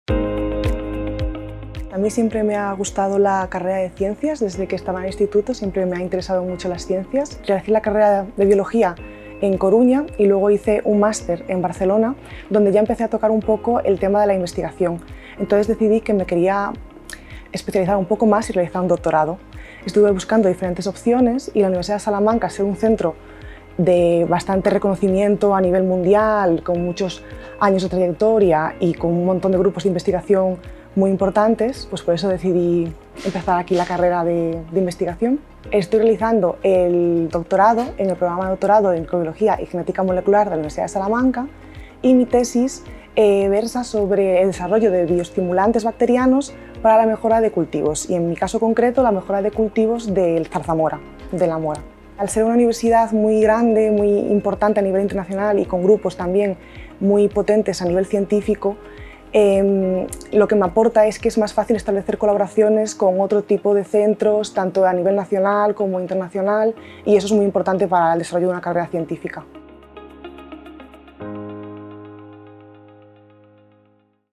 Rocío Roca investiga sobre bioestimulantes bacterianos para la mejora de cultivos.
Rocío Roca Couso está realizando el Programa de Doctorado en Microbiología y Genética Molecular.